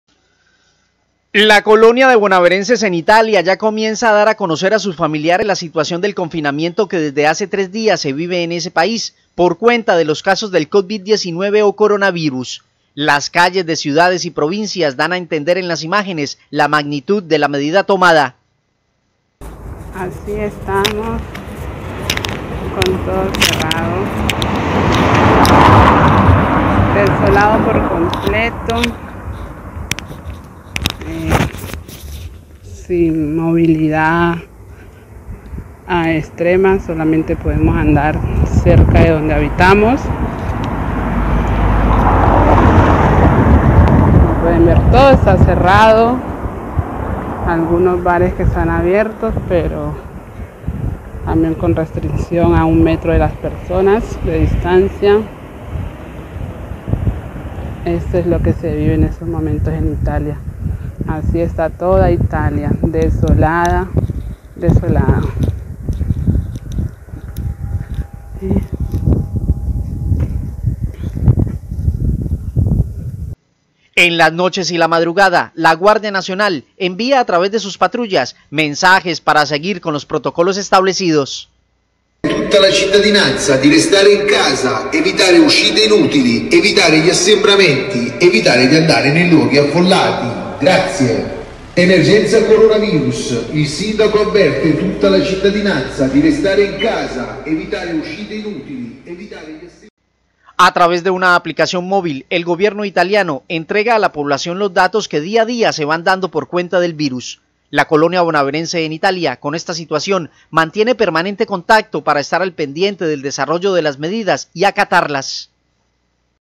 PORTEÑOS EN ITALIA
Colonia de Bonaverenses en Italia dan a conocer como están bajo la medida de confinamiento para evitar avance del convid 19.